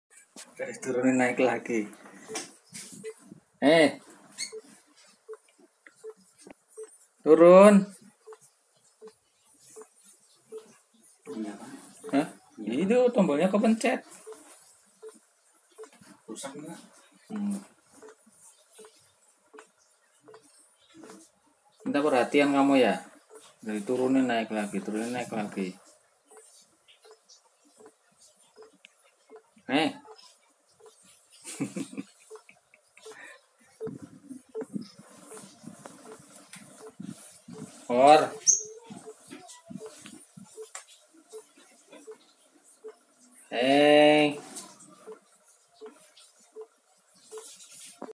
Funny Cat Video | SO FUNNY CAT !!! NAUGHTY CAT ( Si Bawor Tukang Ganggu Kerja )
FunnyCatVideo kali ini tentang Bawor yang mengganggu saat saya sedang kerja Guys. waduhhhh

funny cat videos 2021. funny cat clips,